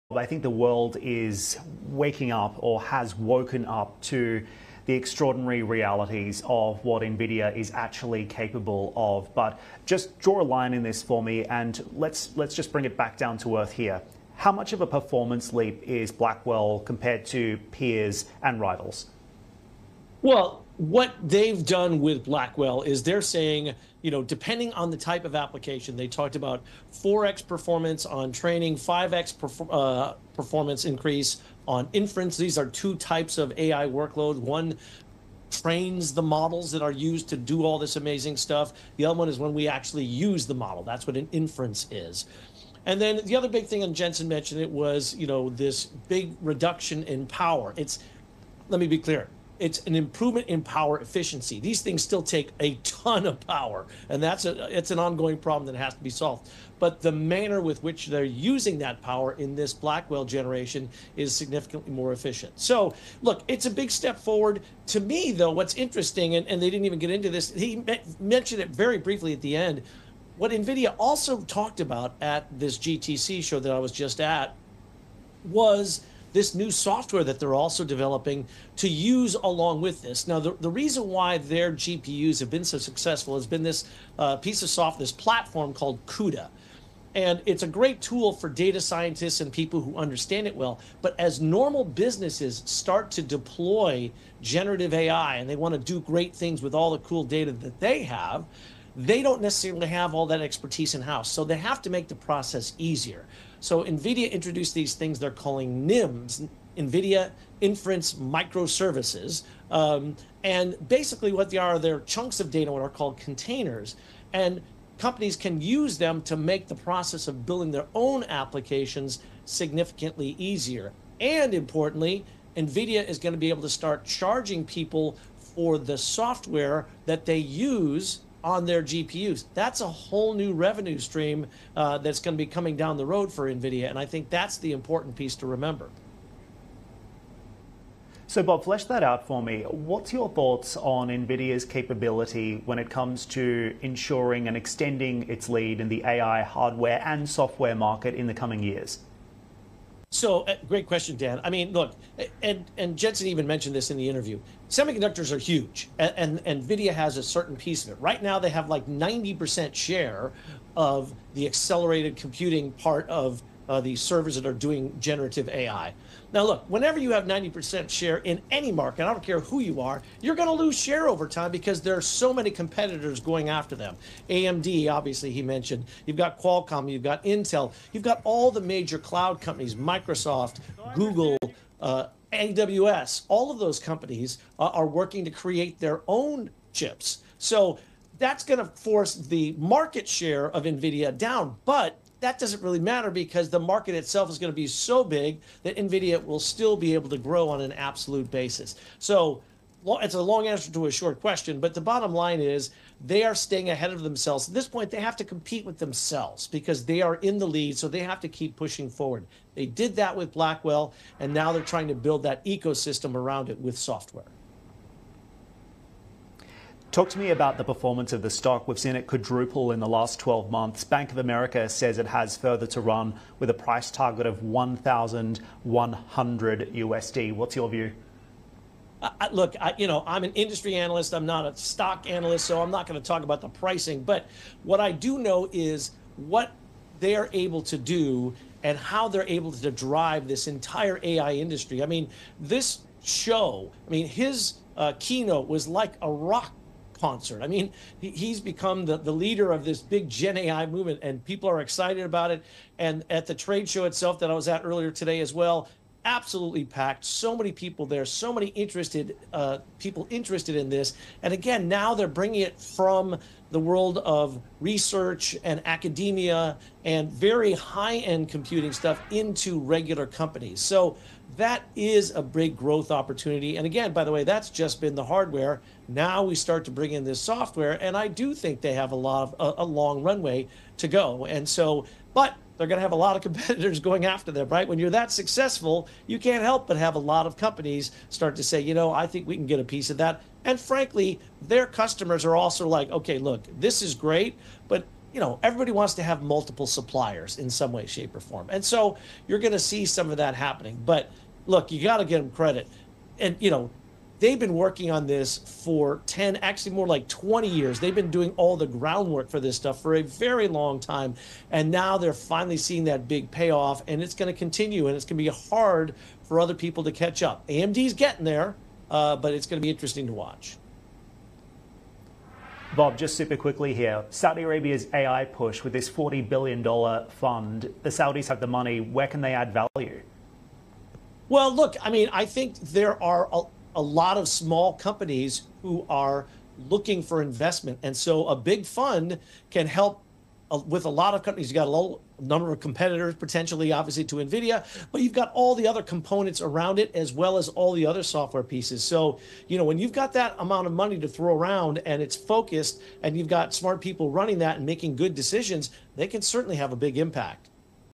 Nvidia will have to compete with itself, analyst says
Bob O'Donnell, president and chief analyst at Technalysis Research, discusses the size of the artificial intelligence chip market and how Nvidia compares with its competitors.